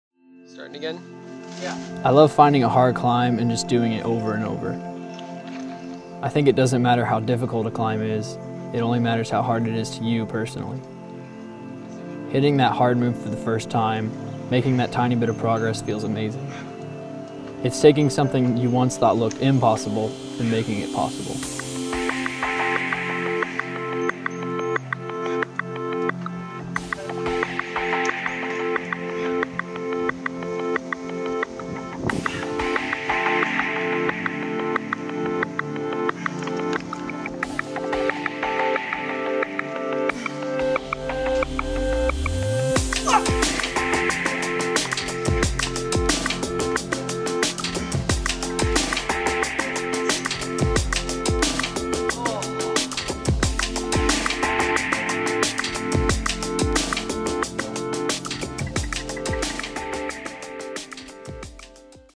Making It Possible // The Whipping Post (V5)
Casen gets the send of Austin, TX Barton Creek Greenbelt "The Whipping Post".

Consider checking out our blog!